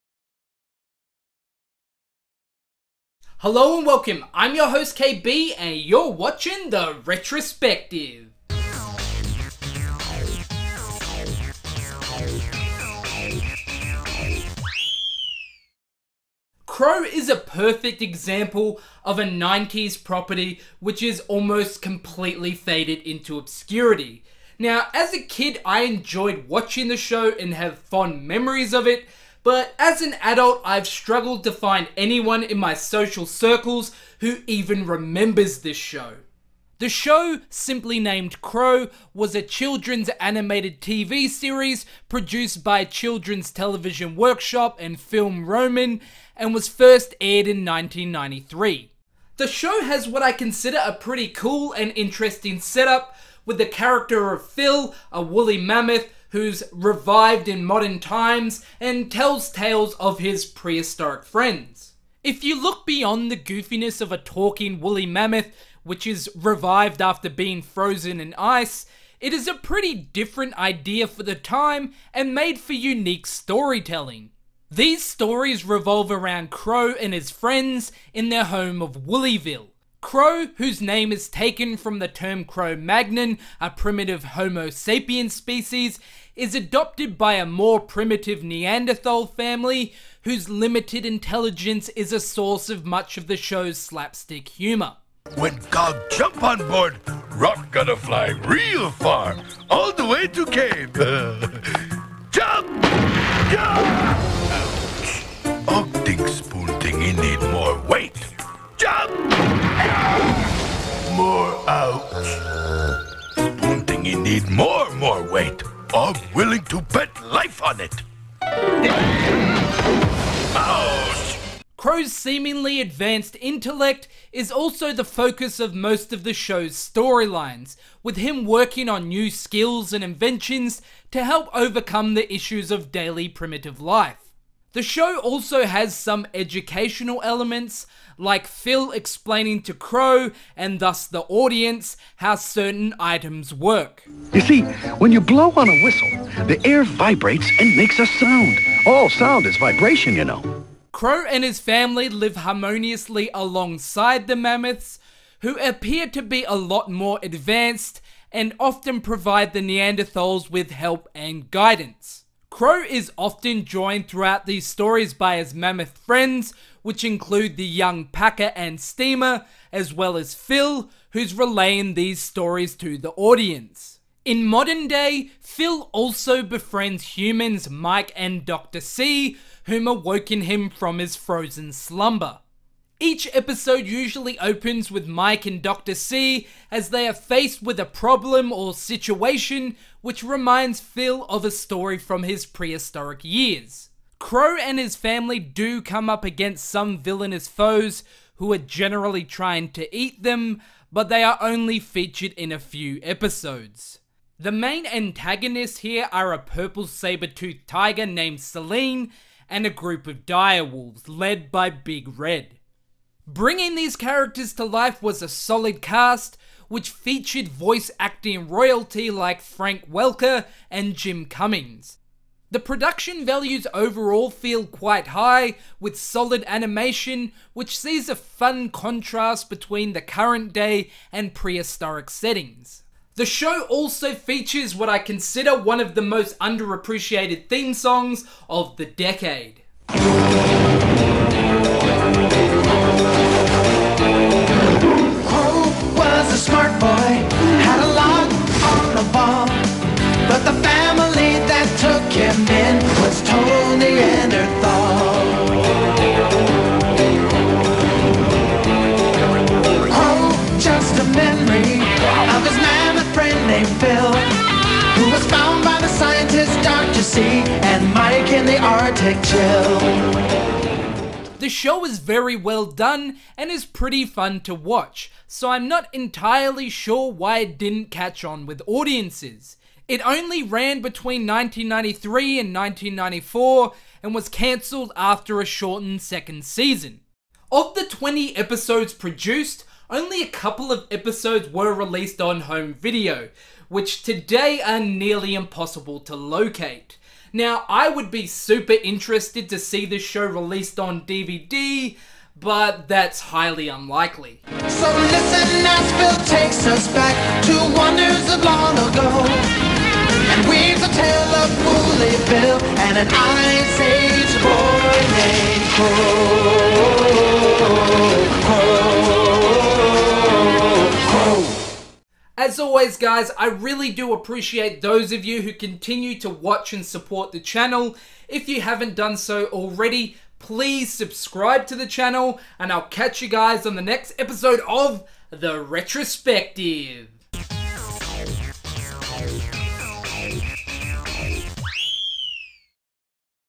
Cro - KB's Retrospective
Going pretty obscure in this one and covering the mostly forgotten 90's animated series Cro